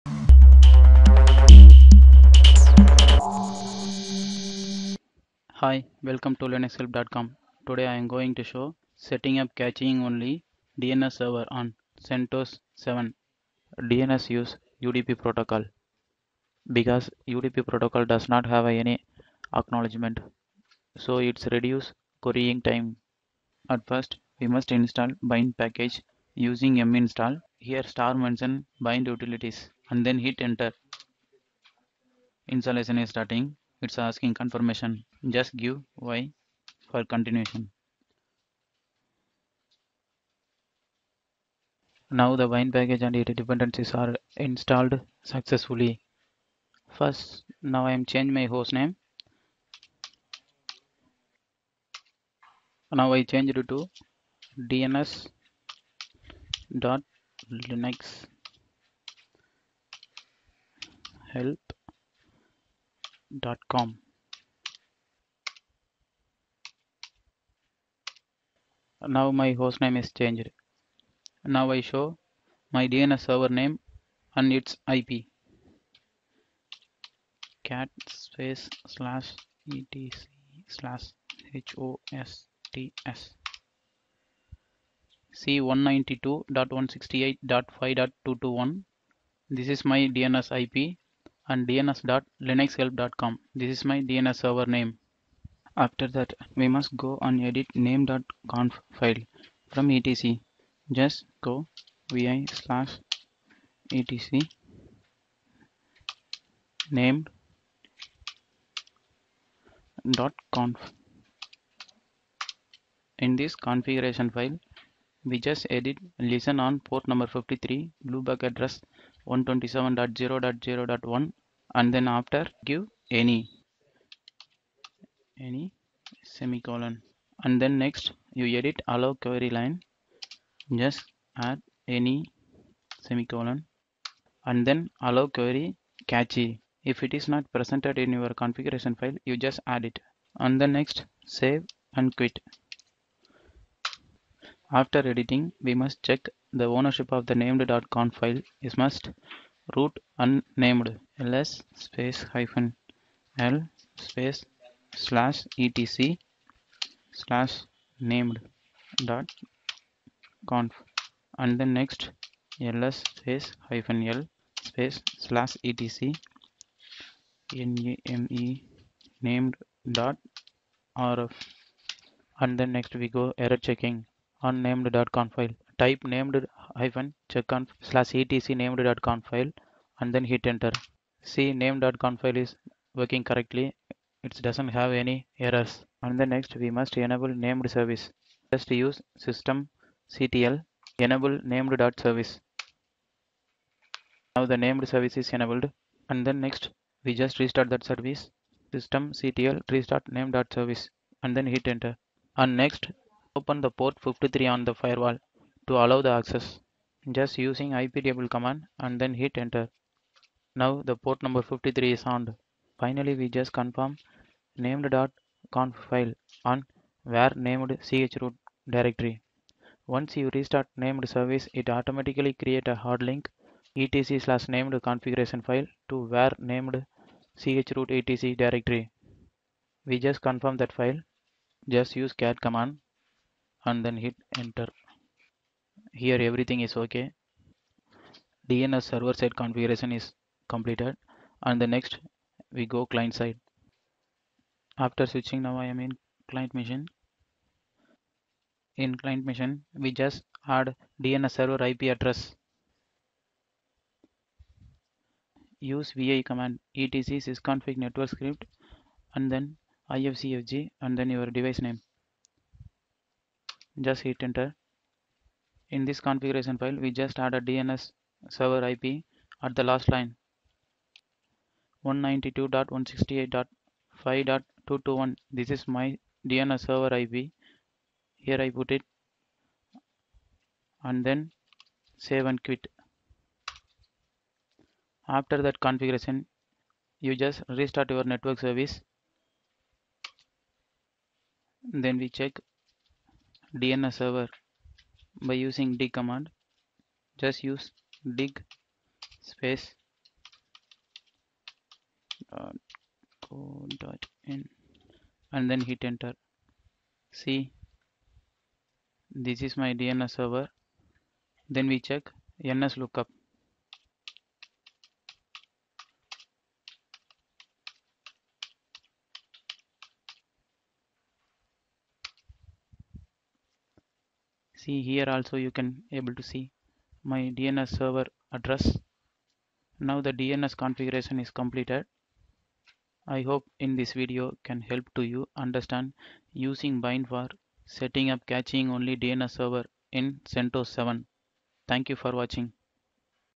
How to setup cache only DNS server in CentOS 7
This video illustrates the methods to set up caching only DNS server using "Bind" in CentOS 7.